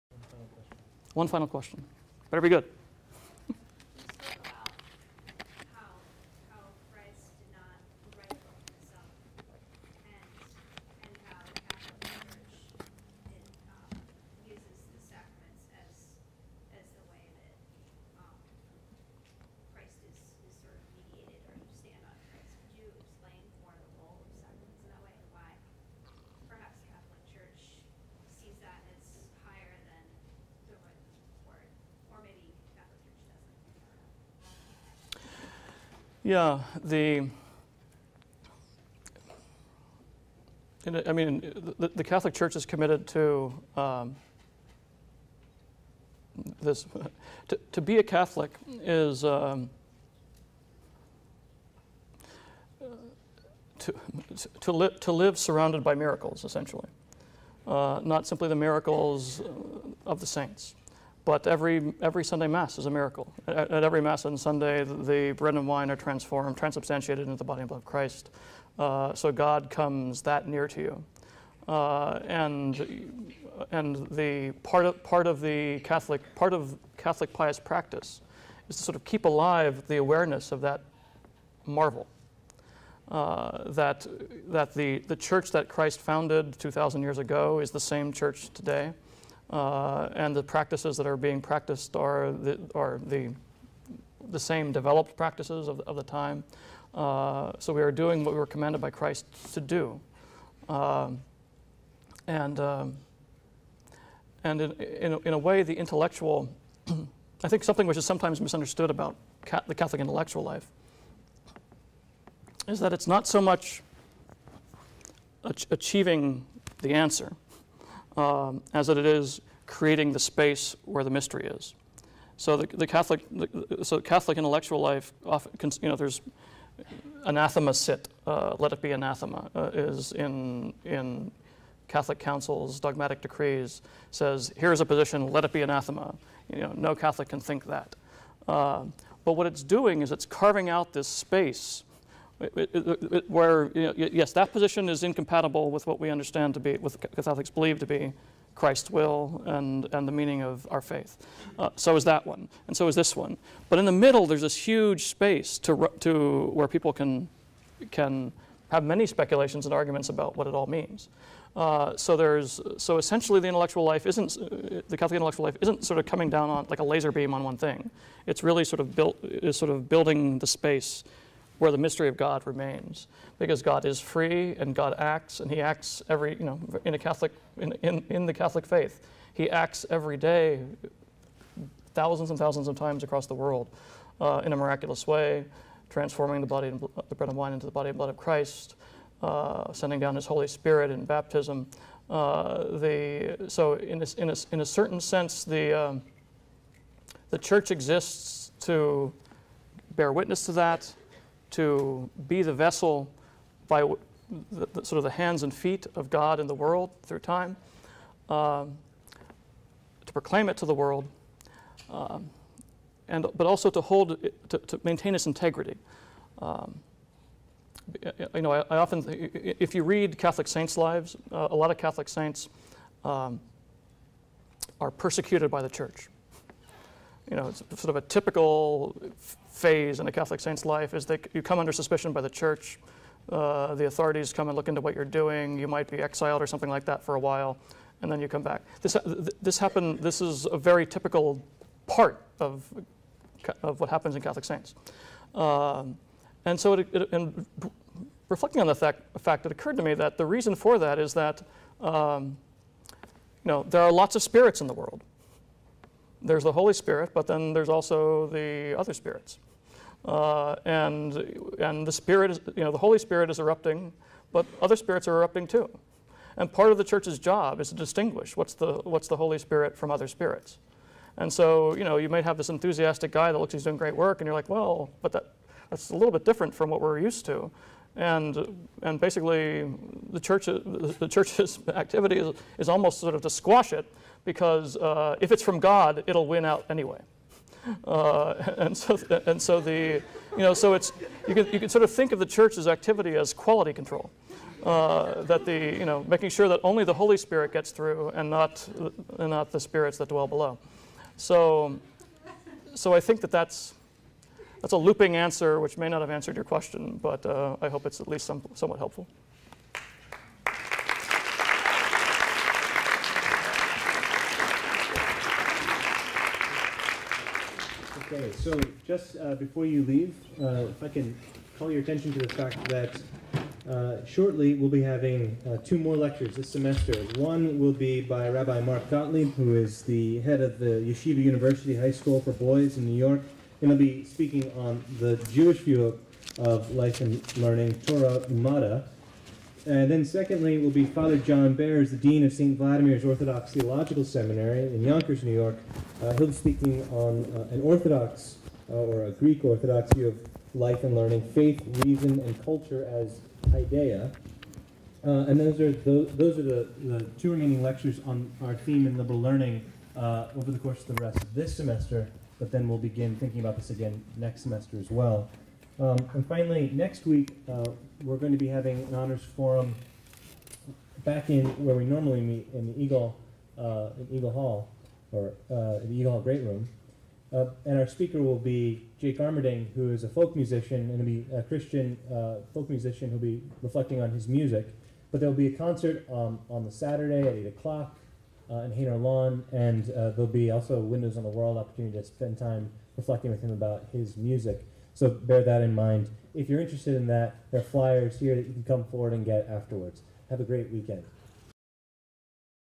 The Roman Catholic View, by Mark Henrie - Part 8
Mark Henrie, Intercollegiate Studies Institute Vice President and Chief Academic Officer will be lecturing on the topic "The Roman Catholic View of Life and Learning: Reality, Incarnation and Beatitude" at 3pm on Friday, September 17 in the McInnis Auditorium. This lecture will be the second of a ten-part series on "Life and Learning in the Great Christian Traditions". This series is a part of the Templeton Honors College public forum offerings. Mark Henrie studied at Dartmouth College, Cambridge University, and Harvard University. For two years he was a novice of the Toronto Oratory, a Roman Catholic religious order. Mr. Henrie has been a contributor to Commentary and First Things, a guest on Mars Hill Audio. He is the Executive Editor of Modern Age and the Political Science Reviewer, is the author of A Student's Guide to the Core Curriculum (2000: ISI Press), and the editor of Doomed Bourgeois in Love: Essays on the Films of Whit Stillman (2001: ISI Press). On the Board of Directors of Regina Luminis Academy in Downingtown, PA. A founding Trustee of the center for European Renewal The Hague, Netherlands.